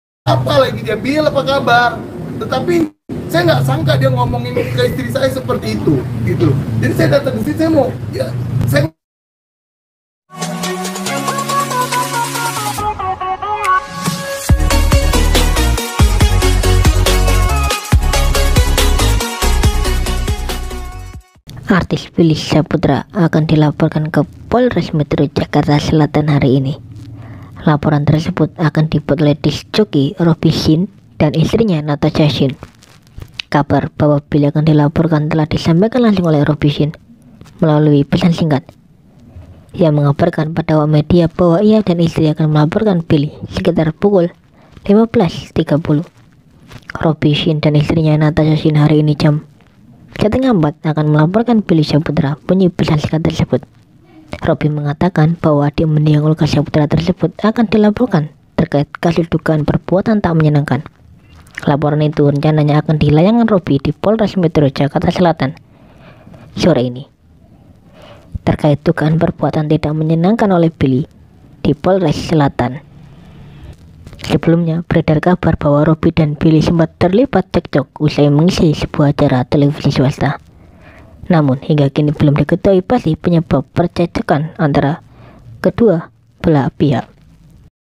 BREAKING NEWS❗BILLY SYAHPUTRA DILAPORKAN KE POLISI SAMA ROBBY SHINE
BREAKING NEWS❗BILLY SYAHPUTRA DILAPORKAN KE POLISI OLEH ROBBY SHINE